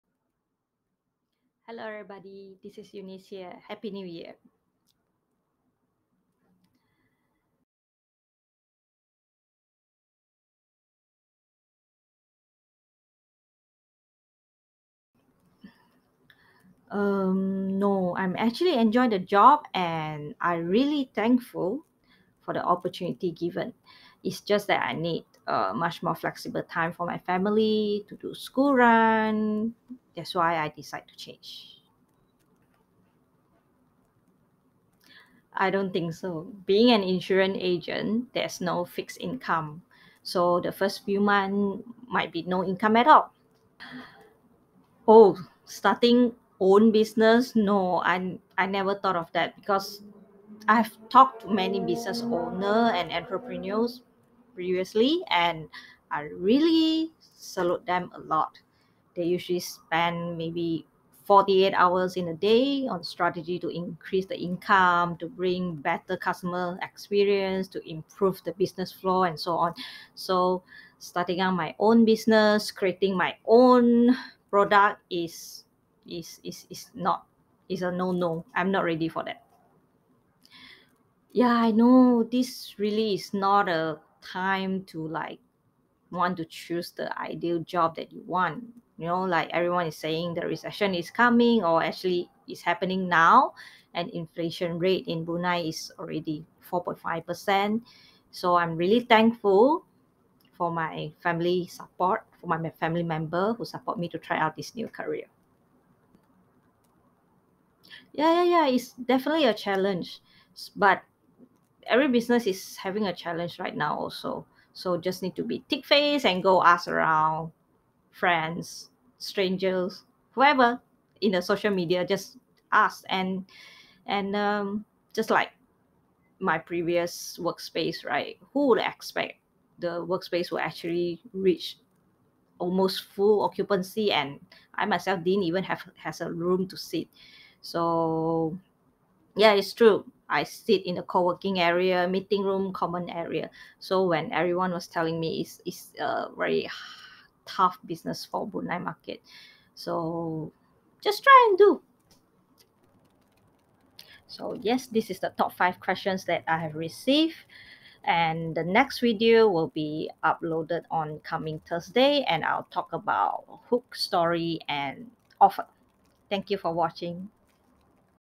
5 questions I received after quit from corporate ( brunei version)
Q1: Not happy with your current job?
Q2: Better offer?
Q3: Why don’t you start your own business?
Q4: Seriously?! A lot of people are jobless now, you know?
Q5: Brunei market so small, and so many agents already oh.